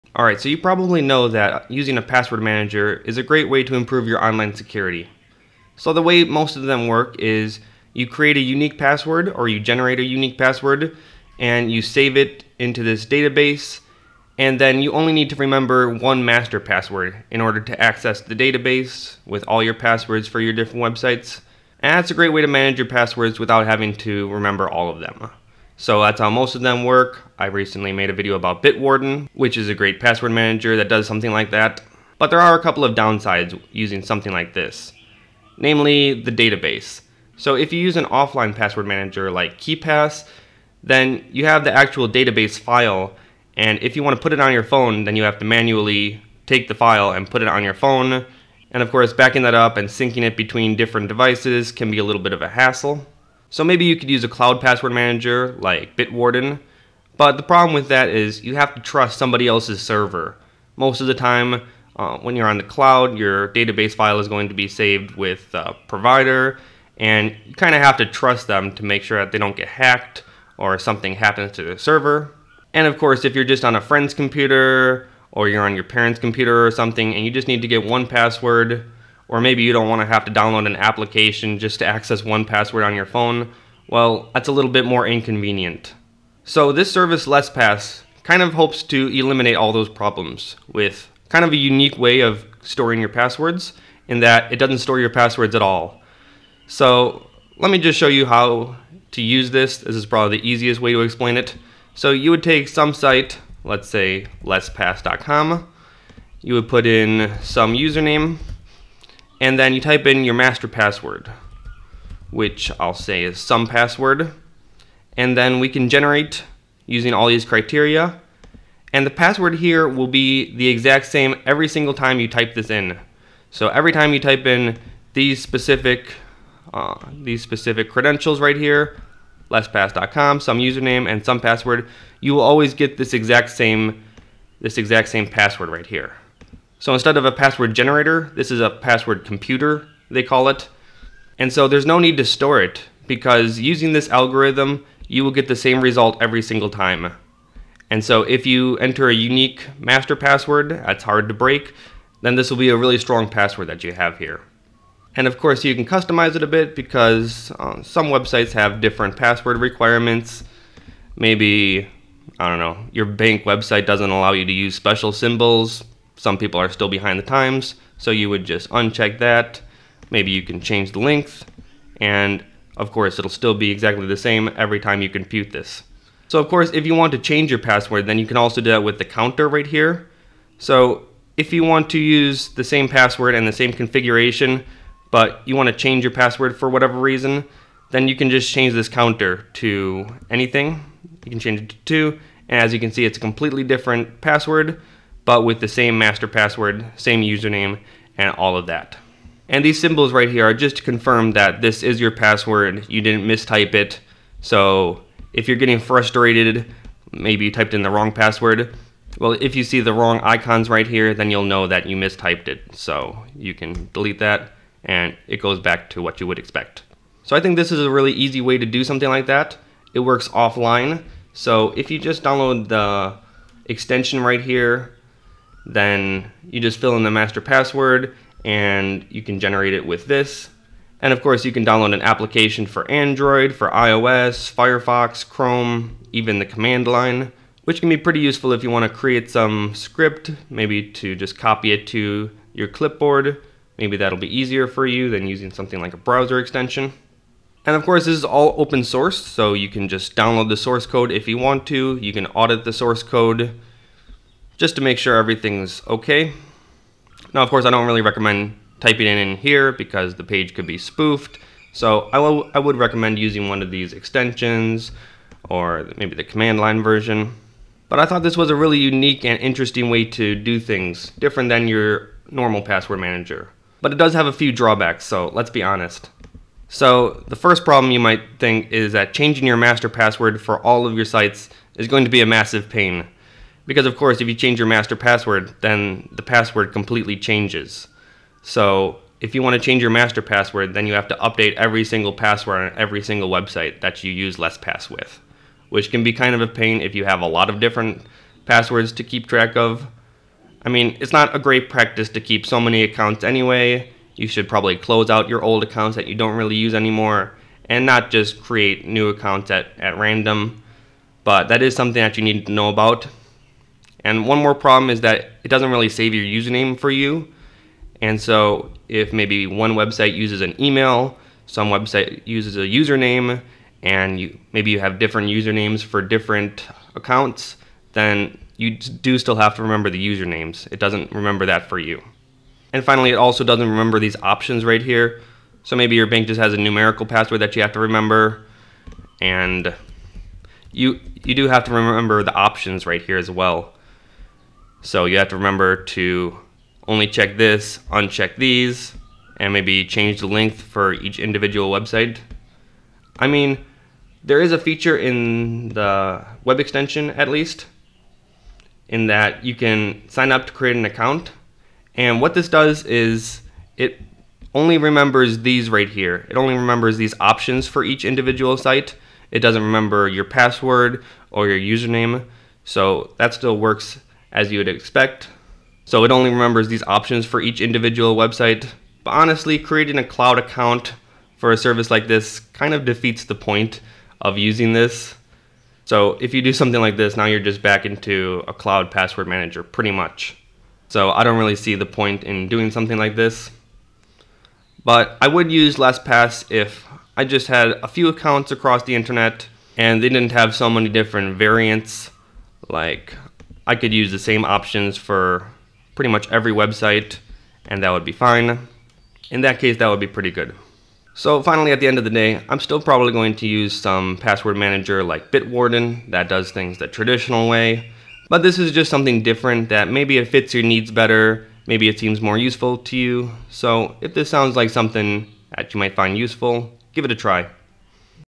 Password Management Without a Database? (LessPass)
Tired of having to sync your database file from device to device with something like Keepass? Don't trust a cloud provider like LastPass or 1Password to keep all of your passwords safe? Try LessPass, which can create a strong, unique password while not having to store any of your passwords inside a database. Now you can finally login to your accounts on your Grandma's computer...